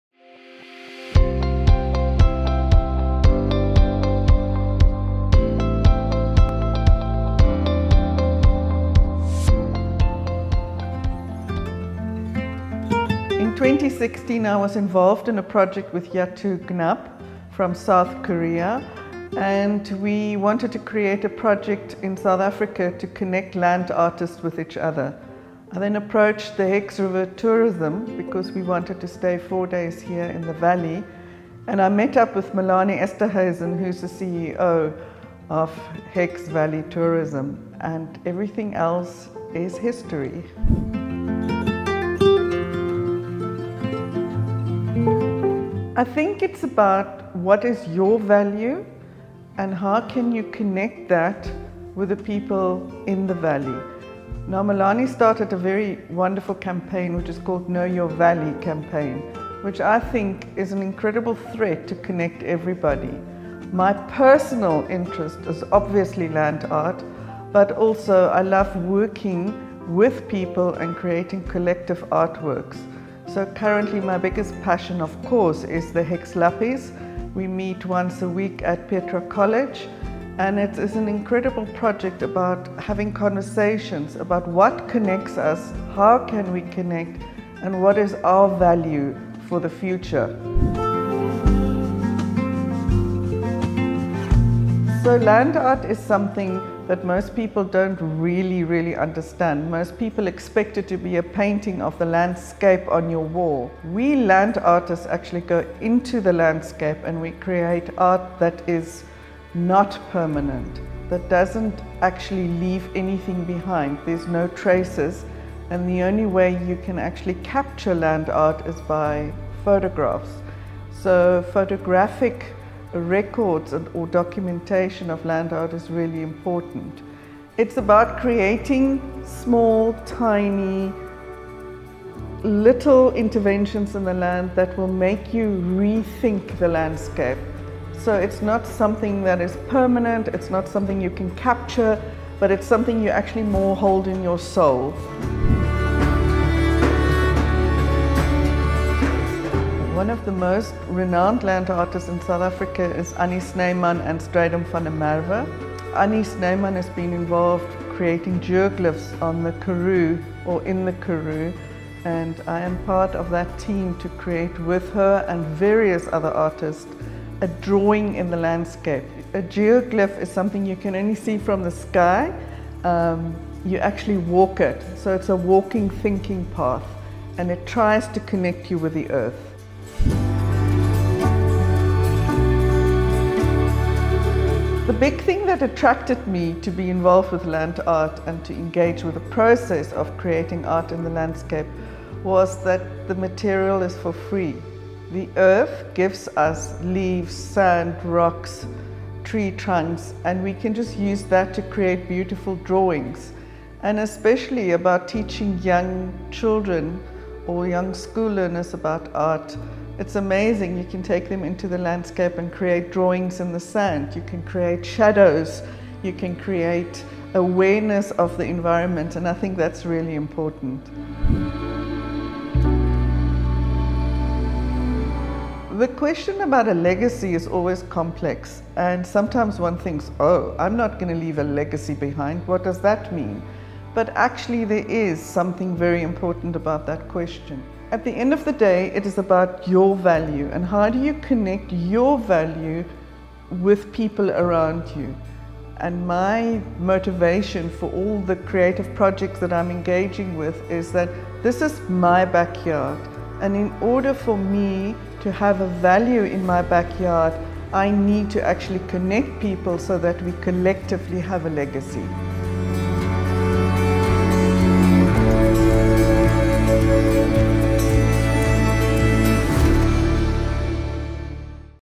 Be The Change by Erica Lüttich
Despite numerous uncertainties caused by the pandemic, the hope and steadfastness of our local brand ambassadors only grew stronger.
Listen how they share their involvement in the local tourism industry and the legacy they hope to leave behind.
Today you'll be listening to local Land Artist, Erica Lüttich.